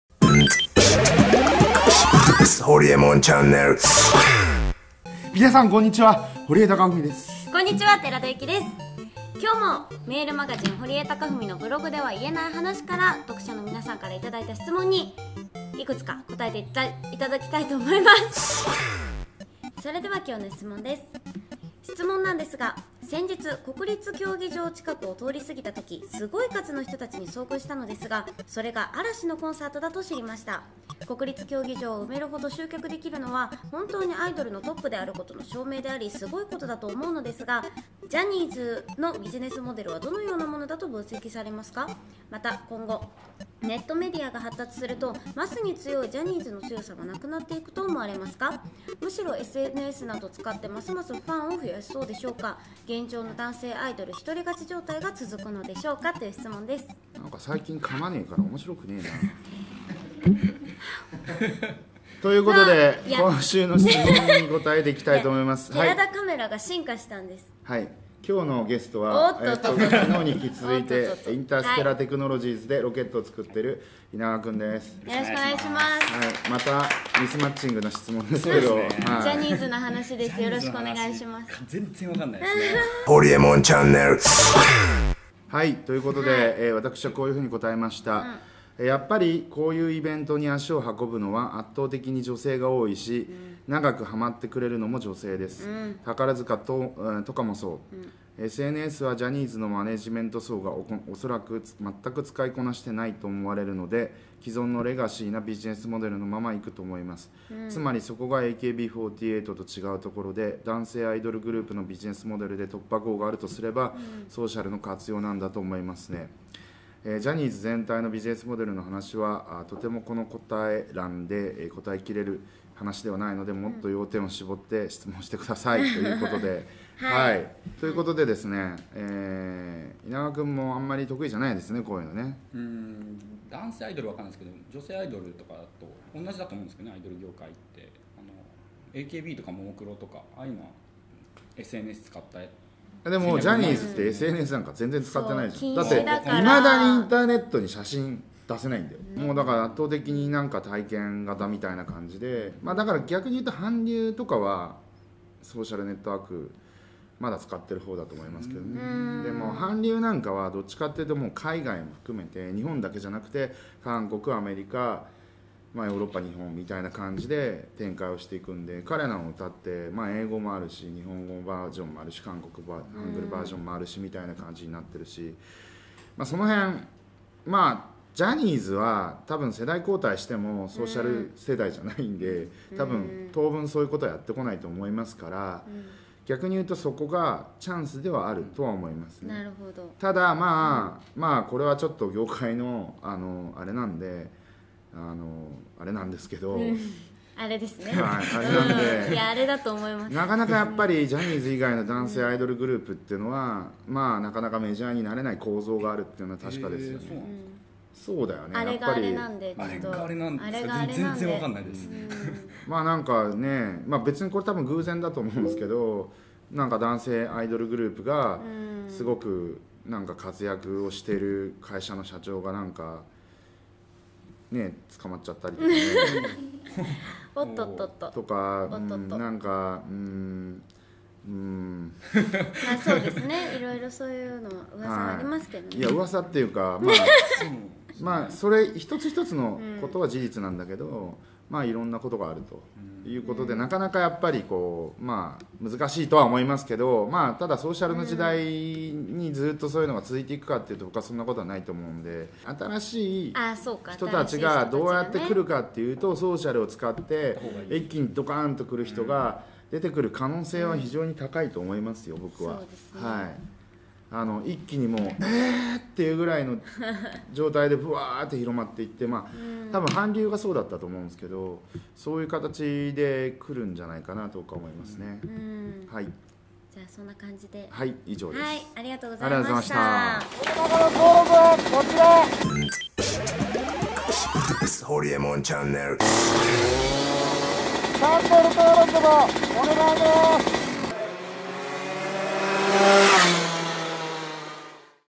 ホリエモンのQ&A vol.38〜ジャニーズのビジネスモデルを分析！？〜
メルマガ「堀江貴文のブログでは言えない話」から大人気コーナーQ&Aが動画になりました！vol.38では「嵐はコンサートで国立競技場を埋める程集客でき、アイドルのトップであることの証明だと思うのですが、ジャニーズのビジネスモデルはどのようなものだと分析されますか。また今後、ネットメディアが発達すると、マスに強いジャニーズの強さはなくなっていくと思われますか？むしろSNSなど使ってますますファンを増やしそうでしょうか？」という質問。ホリエモンの回答はいかに！？今回はホリエモンとロケット開発を行っているインターステラテクノロジズの稲川貴大氏がゲストです！！